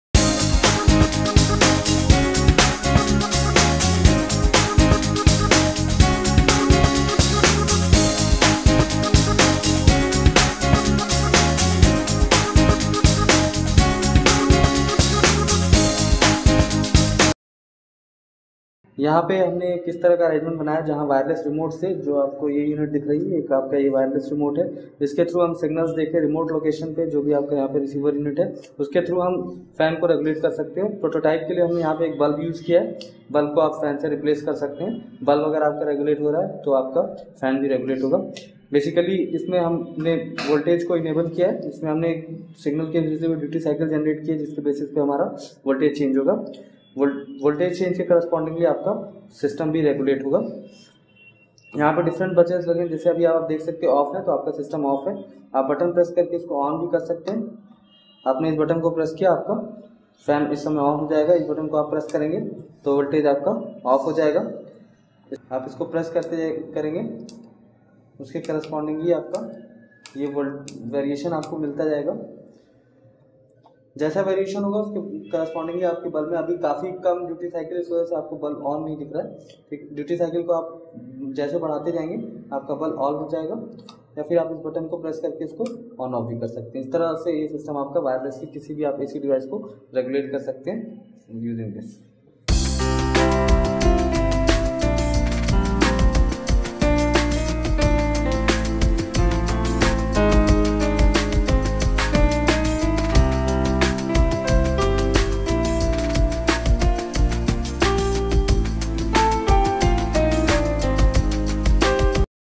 Remote Control based AC Fan Speed Regulator by KitsGuru.com | LGEC072
A household appliance including a fan speed controller , and a method of controlling fan speed of a household appliance, are provided. The system includes a fan speed controller that cut a voltage to the fan motor, measures an electromotive force (EMF) of the fan motor at a predetermined time after the cutting of the voltage to the fan motor, and compares the measured electromotive force (EMF) to a table.

In this kit I am using this same principle to vary the speed of any 1 phase AC motor by varying phase angle of applied AC waveform. The phase angle is varied using DIAC and TRIAC only but DIAC and TRIAC are triggered by digital means using 8051 micro controller.

There are two sections of this system; one is transmitter end and other one is receiver end. At transmitter end there is an encoder IC which encodes the 4 bit data and transmits it with a wireless device working at 315/434 MHz. At the receiver end RF receiver receives the data with antenna and feeds these data to the decoder IC. The decoder IC decodes the data in 4 bit mode and micro controller checks the data sequence with these 4 bits. Display unit is an LCD of 16x2 sizes. It is used for proper user interaction. According to the control inputs pressed on the remote the control parameters that are for regulation is changed and the change is displayed. After this bulb is commanded with remote unit to operate according to set parameter for regulation.